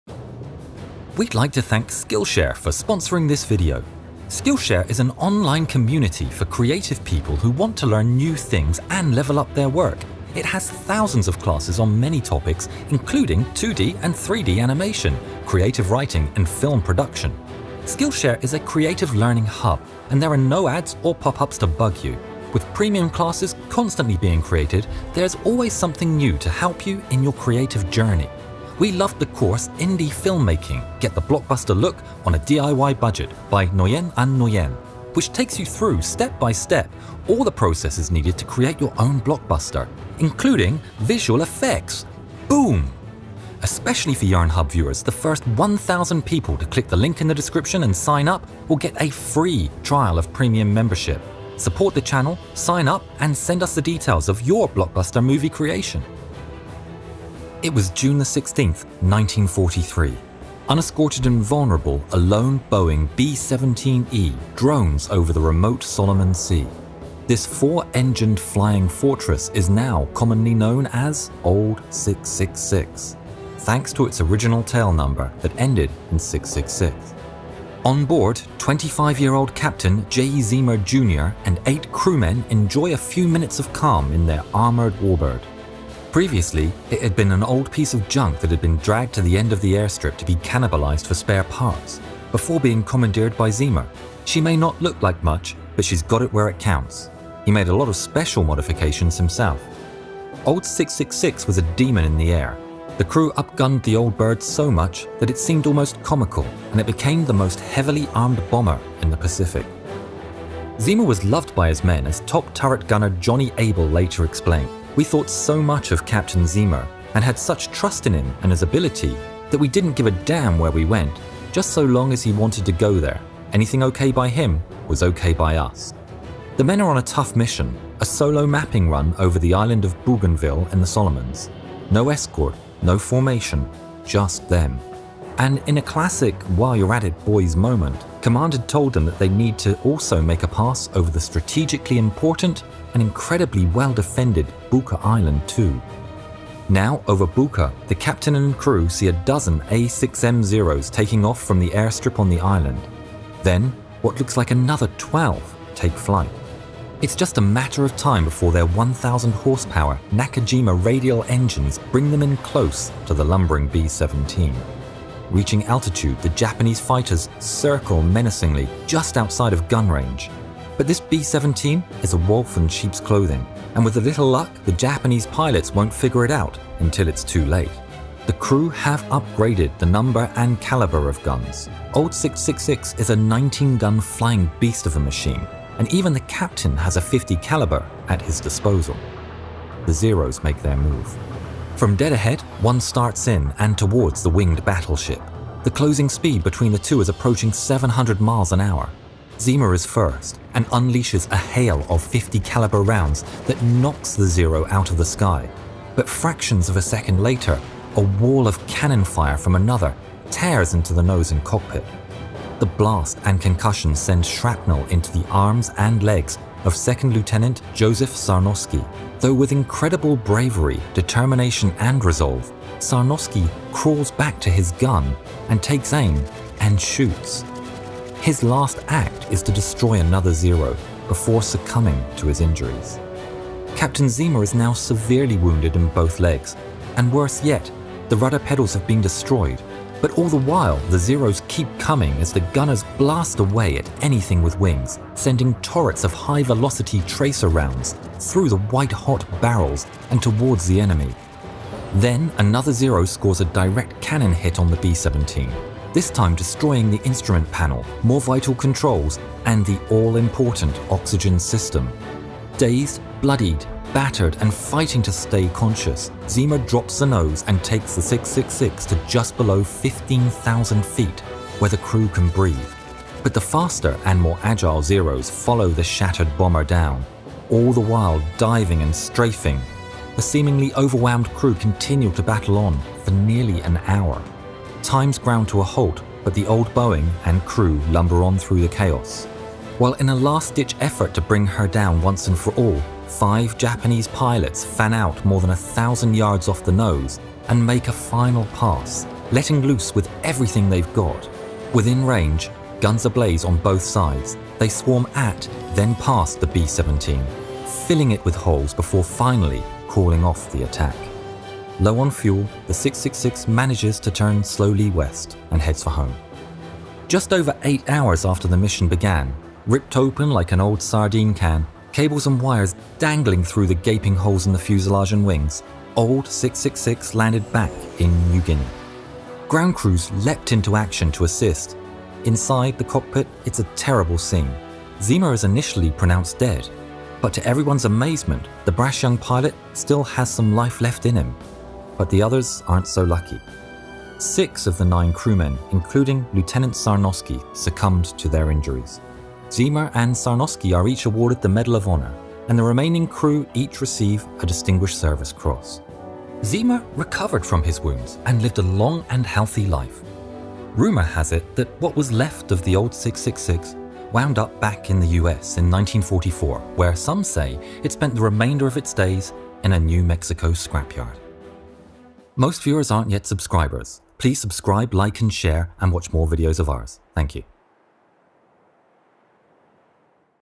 Old 666
Old 666 was a B-17E the number of which ended in 666. It was known as an unlucky plane that had many accidents and mishaps and had even been scrapped for spares. In the end she was rescued by Captain Jay Zeamer and given a number of modifications where it counts… So much so that she couldn't be brought down by dozens of zeros.

Yarnhub uses the Unreal® Engine. Unreal® is a trademark or registered trademark of Epic Games, Inc. in the United States of America and elsewhere.